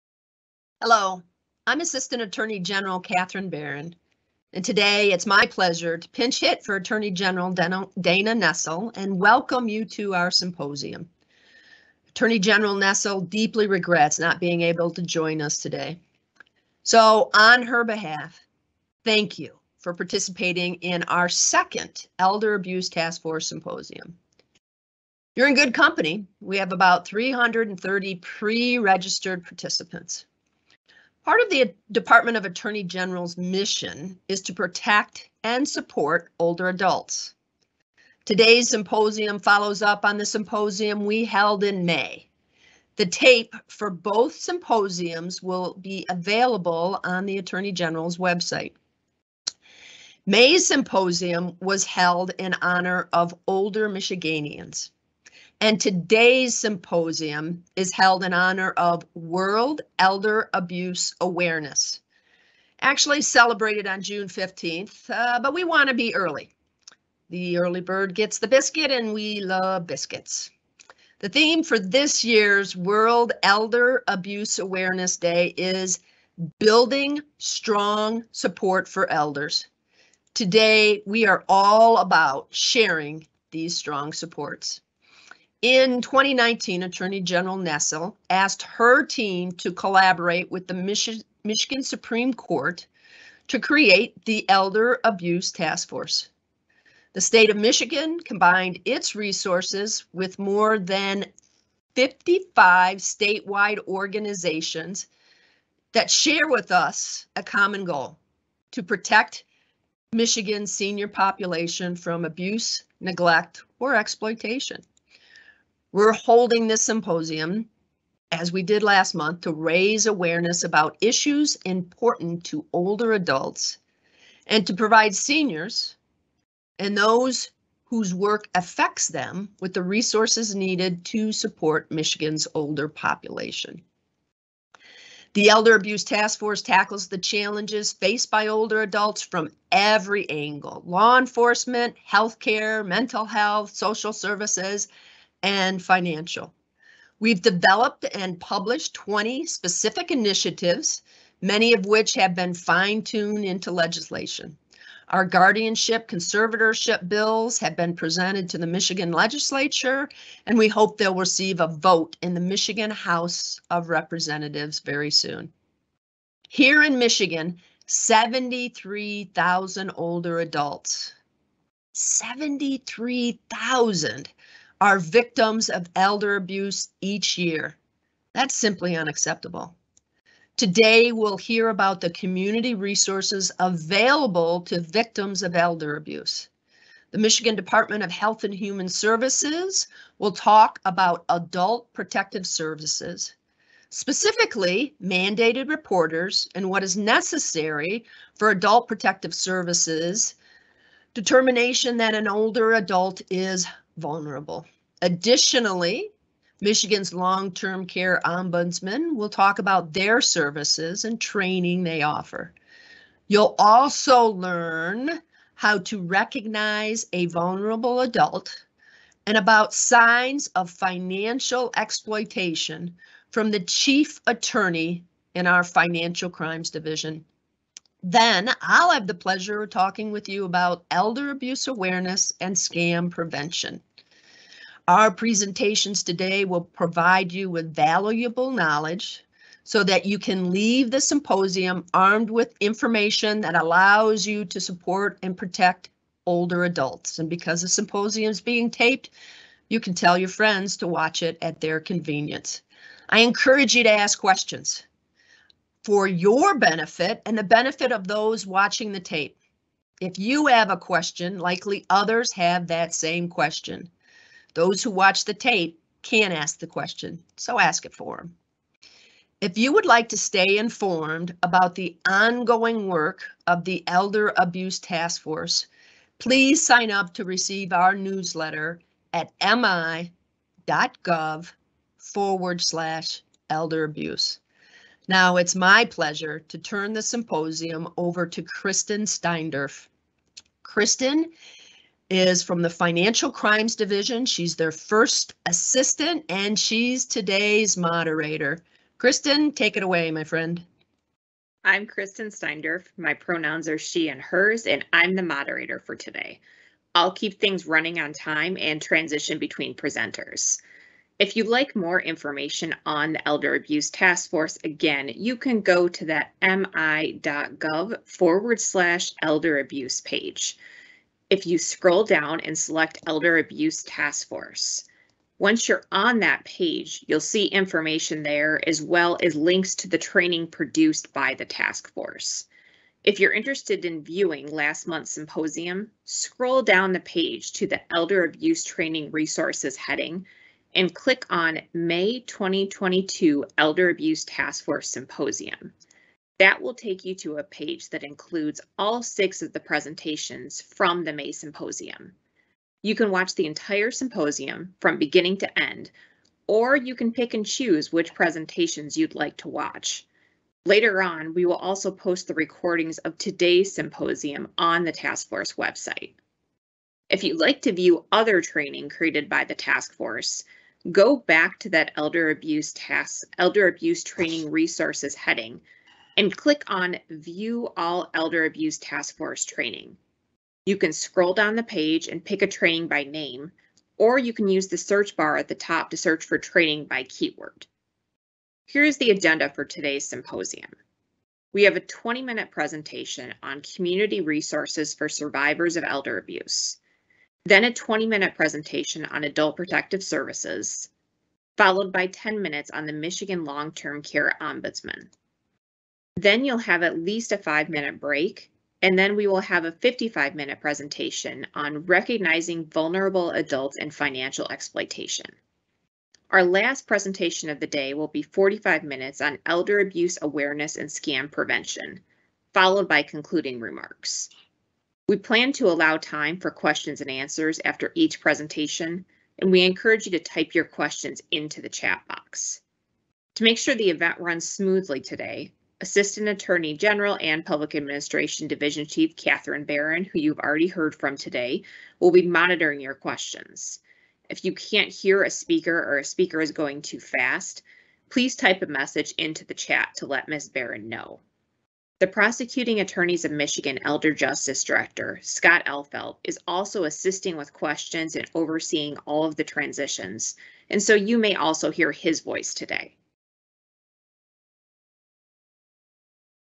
June 2022 Elder Abuse Task Force Symposium Overview
The second Elder Abuse Task Force Symposium follows up on the May symposium and focuses on sharing strong support for seniors and to raise issues important to older adults. Presentations cover community resources available to victims of elder abuse, Adult Protective Services, Michigan's Long Term Care Ombudsman services, how to recognize a vulnerable adult and signs of financial exploitation, and finally elder abuse awareness and scam prevention.